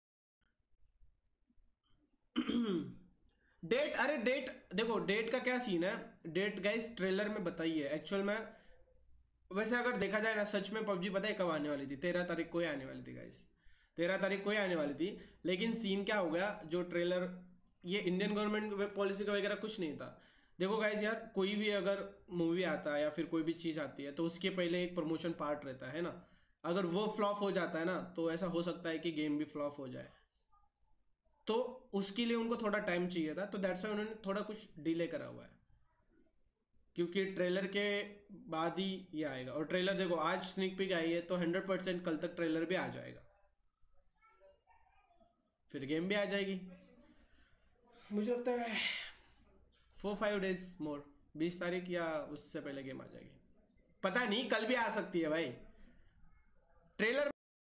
Kronten Talks About - When Pubg in PlayStore ?, Governement issues ?, When Trailer Coming ?
Kronten Talks About

✓ When Pubg Coming in playstore
✓ Governement issues
✓ trailer launch ?